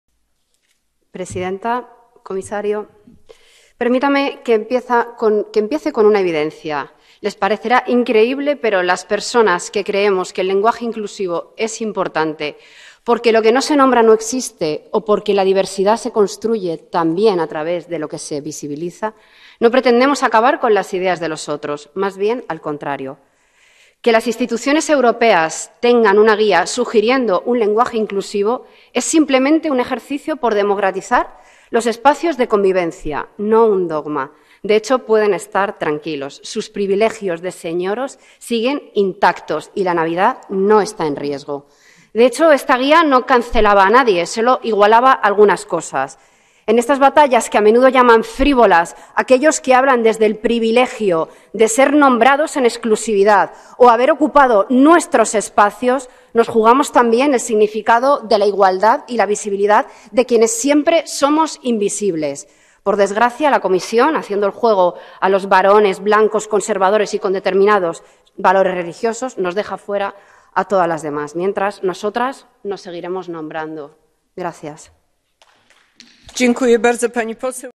Sira Rego: "Derecha y extrema derecha, alianza de señoros que ven peligrar sus privilegios"
Intervención en el pleno del Parlamento Europeo de Sira Rego, portavoz de Izquierda Unida y eurodiputada de Unidas Podemos, durante el debate del miércoles 15 de diciembre de 2021 acerca de las orientaciones de la Comisión Europea sobre lenguaje inclusivo.